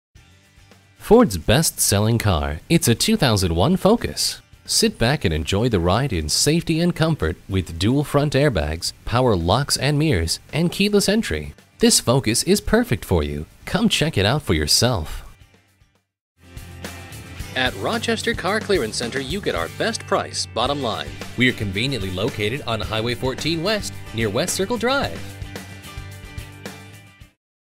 2001 Ford Focus Rochester Winona, MN MA15066 - SOLD
Year: 2001
Make: Ford
Model: Focus
Trim: SE
Engine: 2.0L I4
Transmission: 4-Speed Automatic
Color: Blue
Mileage: 178736
Stock #: MA15066
VIN: 1FAFP34381W379569

Tune-Ups
Transmission Service
Brake Service
Performance Parts
Factory Wheels

Address:
2980 HWY 14